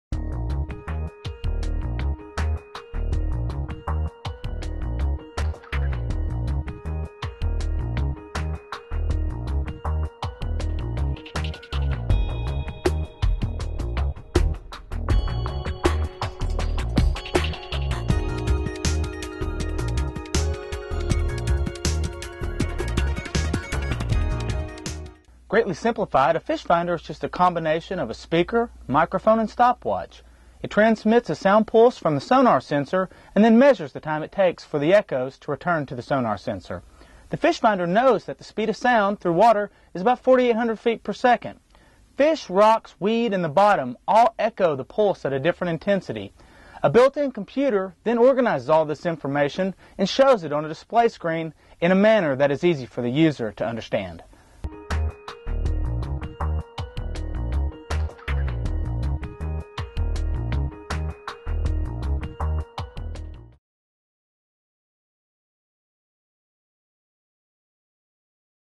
HawkEye® TechTutorial™ | How a Fish Finder Works
This Video Short, from the makers of HawkEye® fish finders and depth sounders, contains a brief video explanation of how a fish finder works.